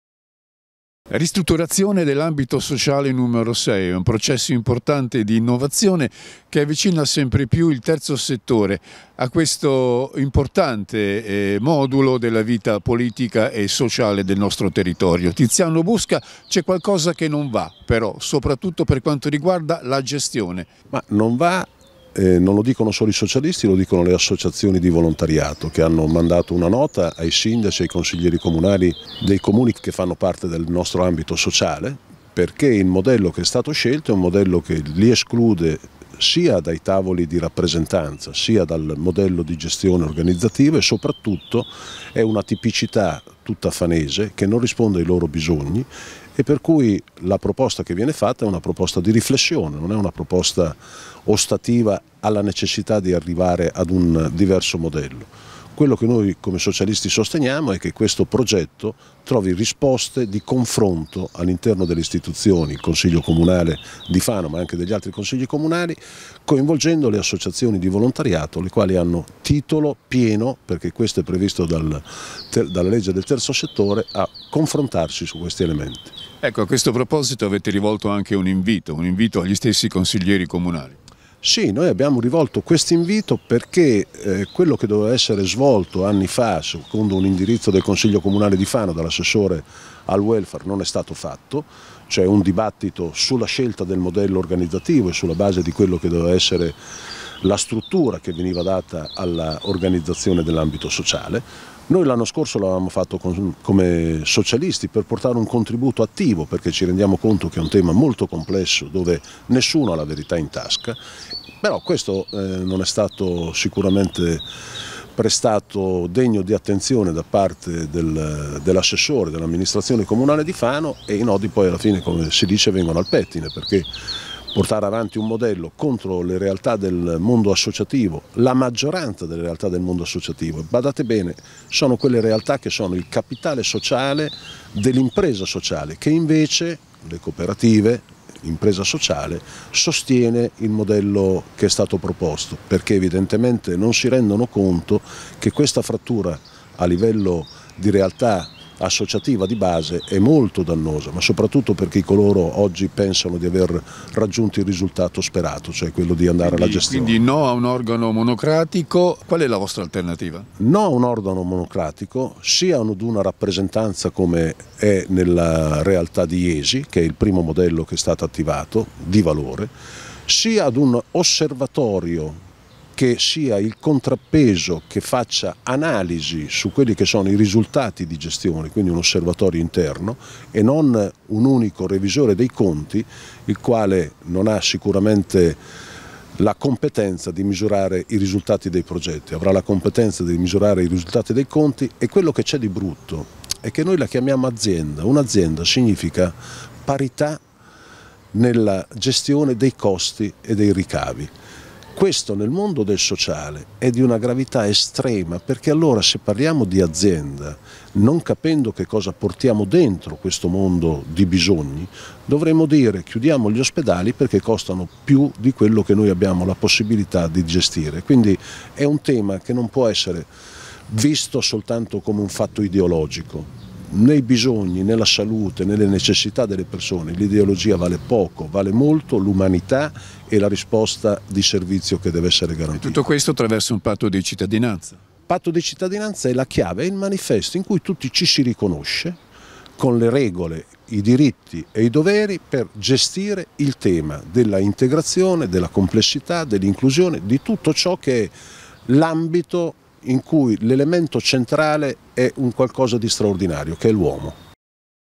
Riforma Ambito Sociale: NO dei Socialisti
La riforma dell’Ambito Sociale tiene alto il confronto politico. La decisione dell’assessore al Welfare Dimitri Tinti, quale presidente della Conferenza dei sindaci, di istituire una figura centrale a cui vengono riservate le decisioni, ha dapprima sollevato la contrarietà del Terzo Settore, poi mosso una serie di critiche da parte di tutte le forze dell'opposizione. I primi a insorgere sono stati i Socialisti.